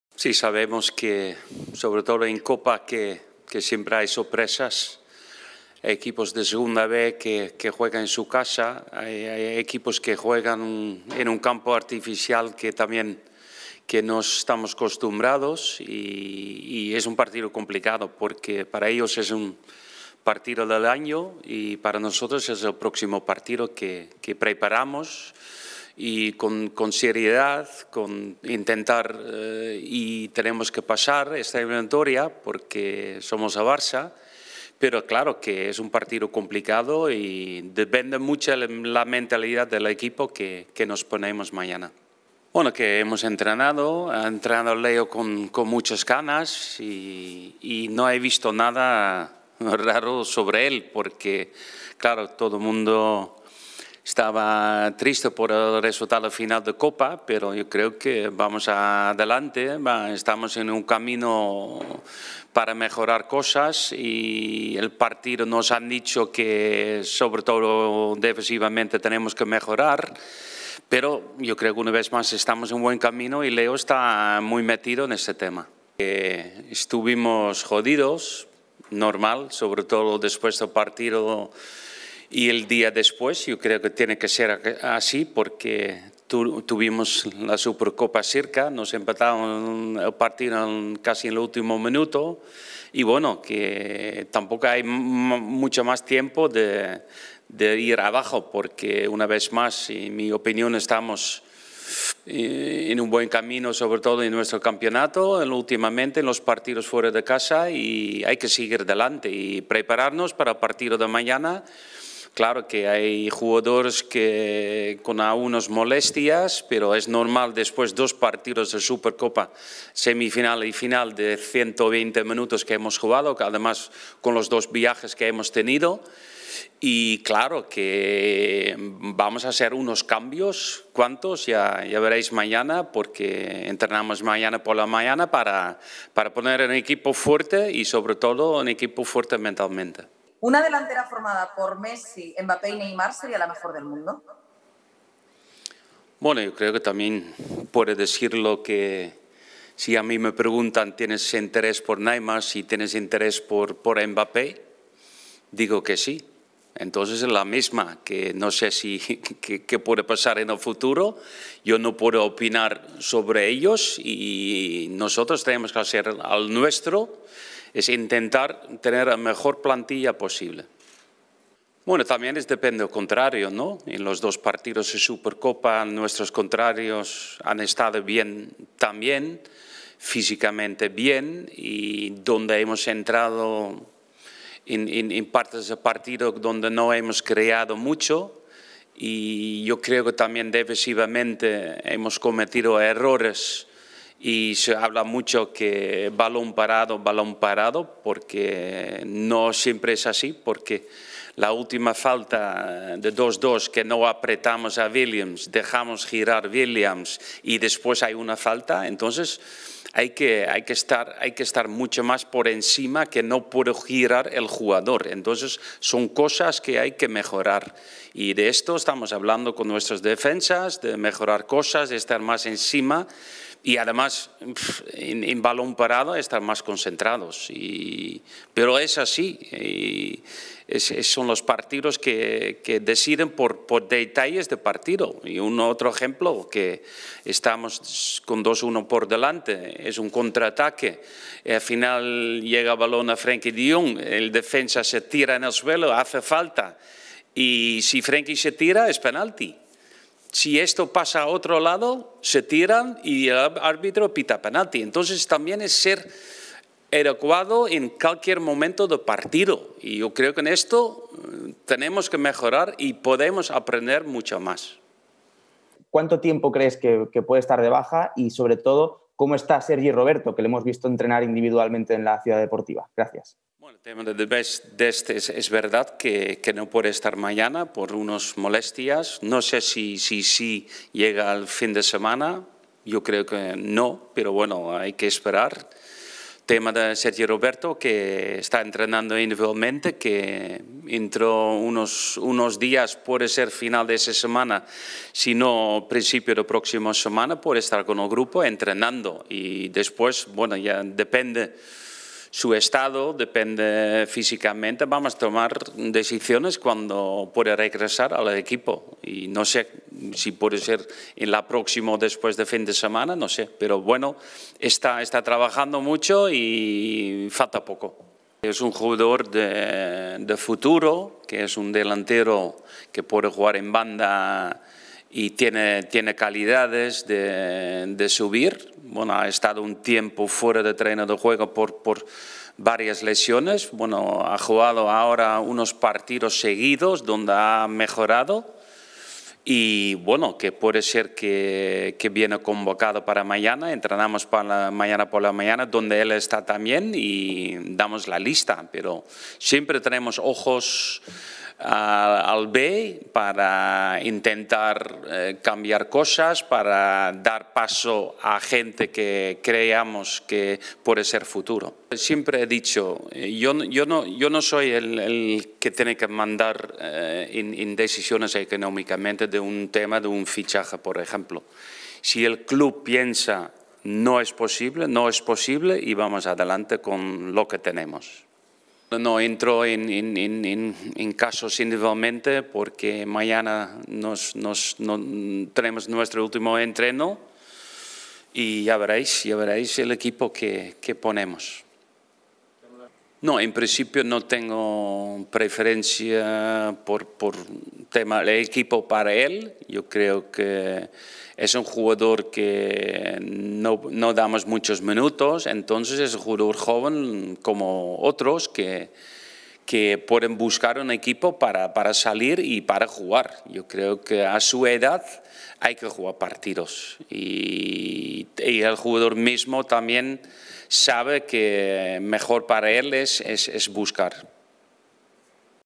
¿Messi al PSG? - Rueda de prensa de Koeman
¿Leo Messi al PSG?
Fichajes del Barcelona
Lesión de Sergi Roberto

🗣 Rueda de prensa de Ronald Koeman previo a la Copa del Rey